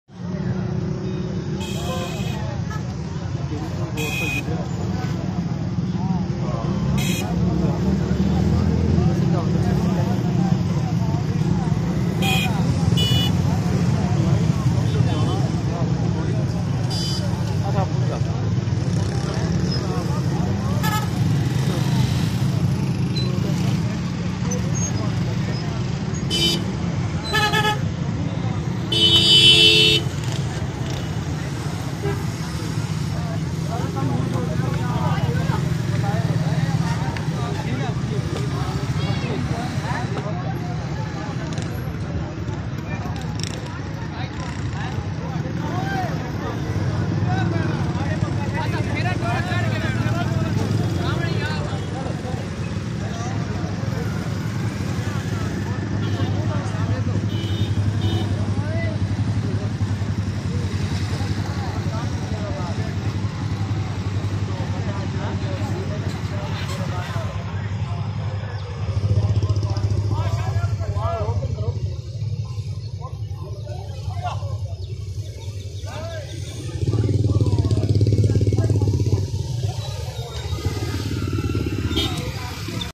Latest Flood position at jauharabad hadali bola awan town police line bridge
Road is blocked and cracks are appearing in bridge at police line